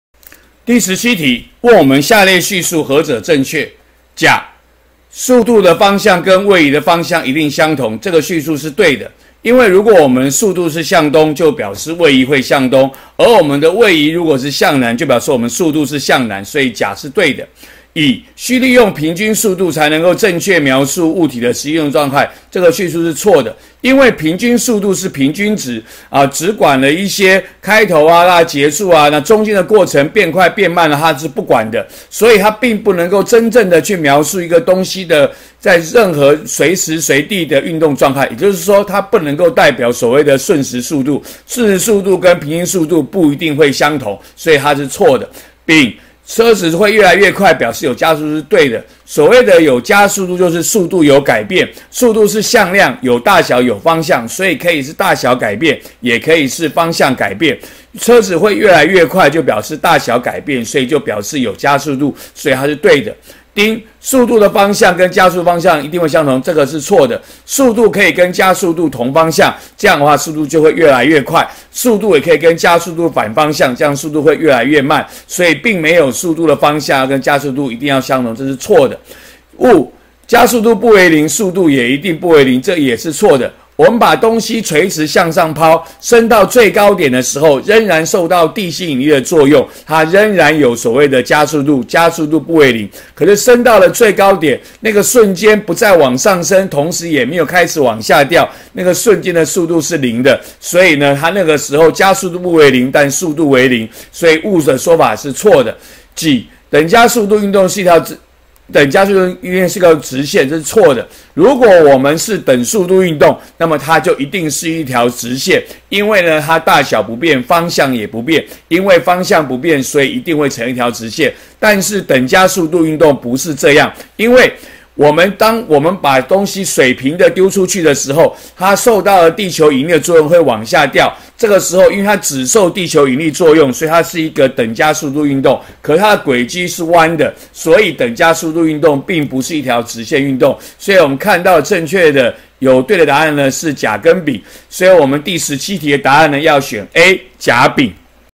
103學年上9年級理化一段第17題
給6個敘述，問何者正確。